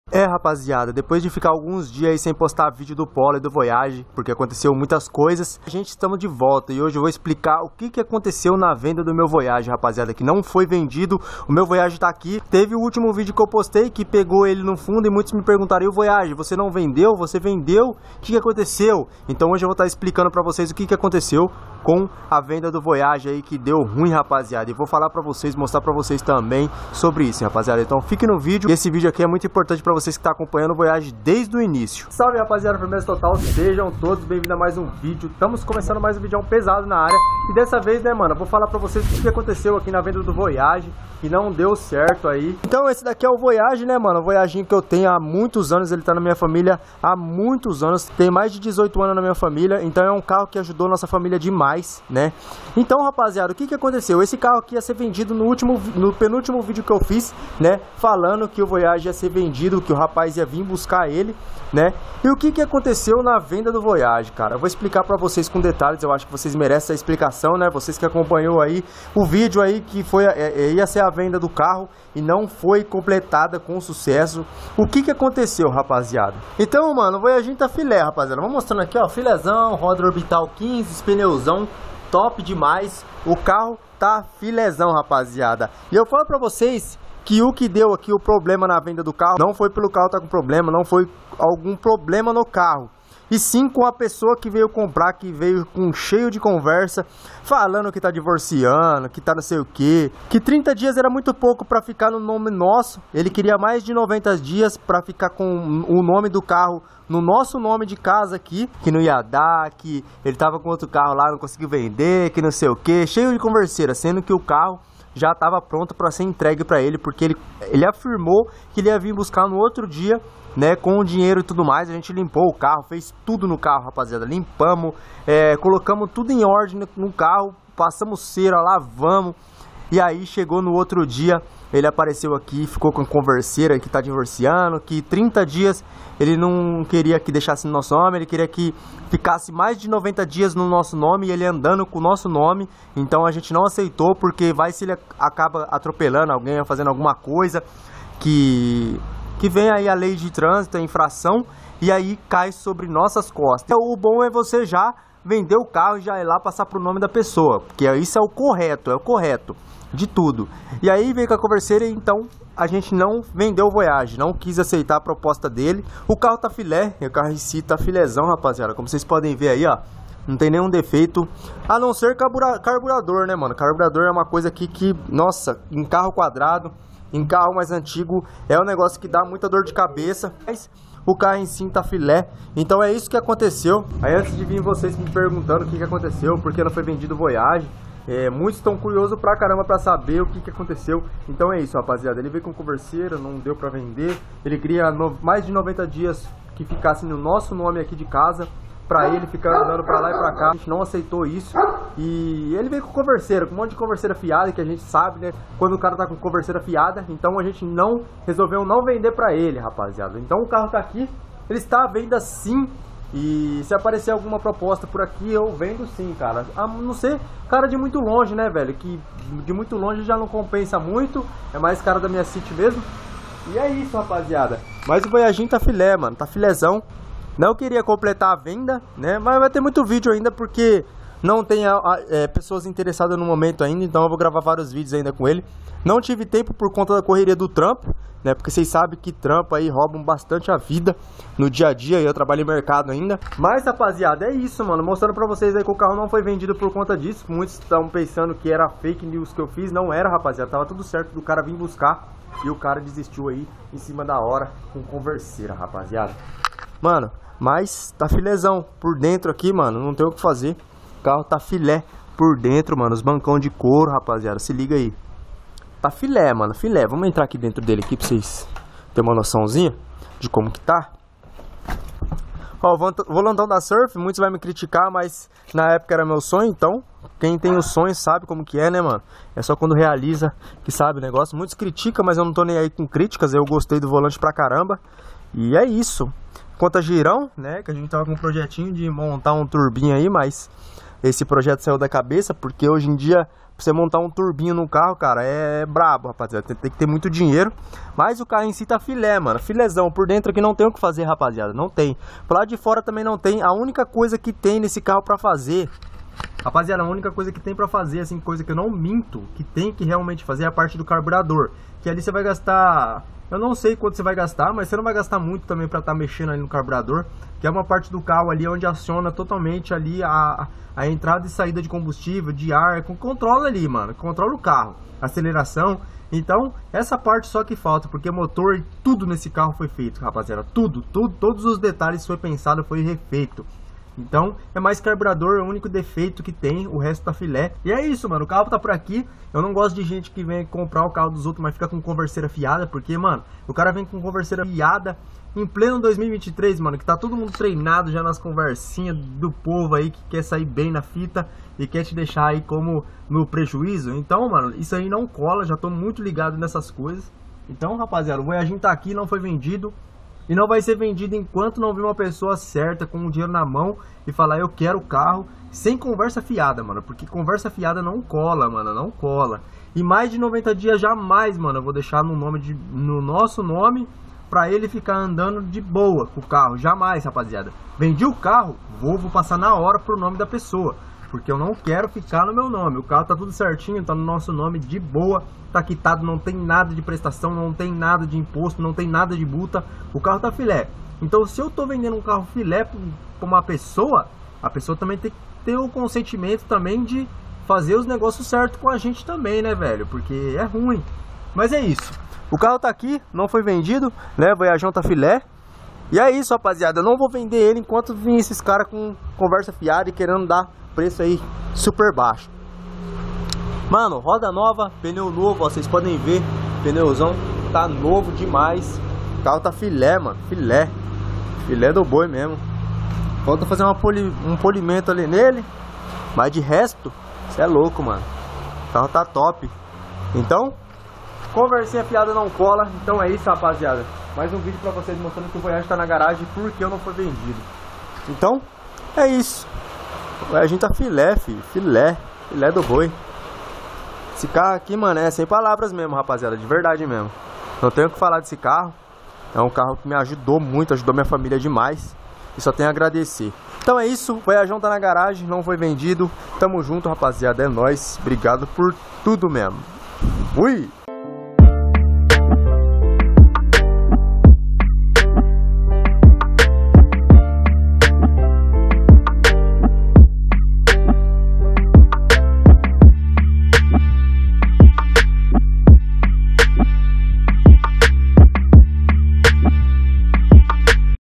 Voyage Nao Foi Vendido QUASE LEVEI GOLPE
Expliquei oque houve na venda do voyage , espero que entendam tudo certinho

► Carro : POLO SEDAN 2008
► Carro : VOYAGE QUADRADO ANO 85
► Carro : FIESTA SUPER CHARGE ANO 2003
► Carro : FORD KA 2009